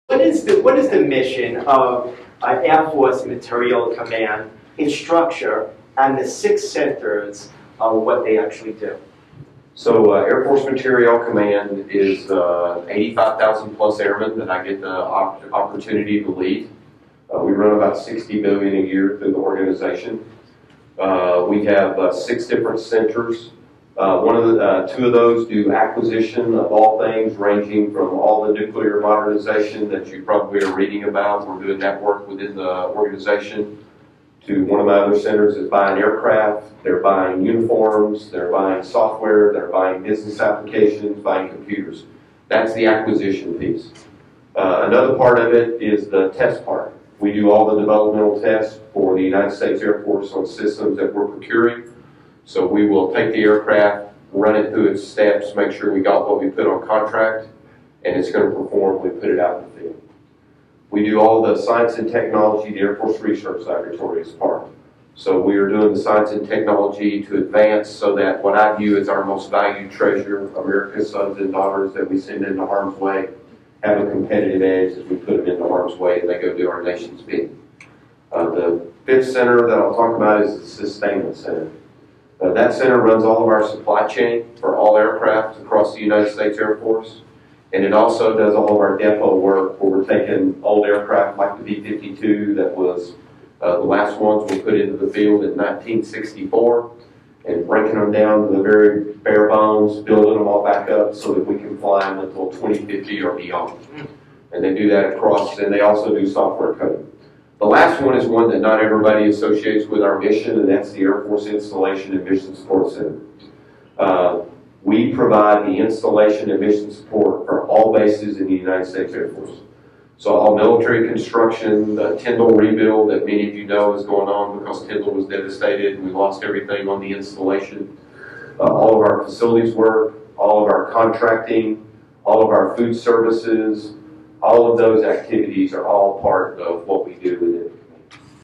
4-Star General Bunch, Commander of Air Force Innovation Talks About His Job
4-star General Arnold W. Bunch Jr. is Commander, Air Force Materiel Command, headquartered at Wright-Patterson Air Force Base, Ohio. He leads future innovation for the Air Force.  He is responsible for installation and mission support, discovery and development, test and evaluation, life cycle management services and sustainment of virtually every major Air Force weapon system. The command employs approximately 80,000 people and manages $60 billion of budget authority annually.

He completed operational assignments as an instructor, evaluator and aircraft commander for the B-52 Stratofortress. Following graduation from the Air Force Test Pilot School, Gen. Bunch conducted developmental testing in the B-2 Spirit and B-52, and served as an instructor in each. Additionally, he has commanded at the squadron, group, wing, and center levels. Prior to his current assignment, he was the Military Deputy to the Assistant Secretary of the Air Force for Acquisition, Technology, and Logistics, at the Pentagon, Arlington, Virginia.

This will be a fascinating event as China exponentially increases its military spending, and the strategic threats worldwide continue to increase.  What does the future U.S. Air Force look like?  What type of aircrafts, systems, and weapons are in development, and are envisioned to keep America, and U.S. troops safe.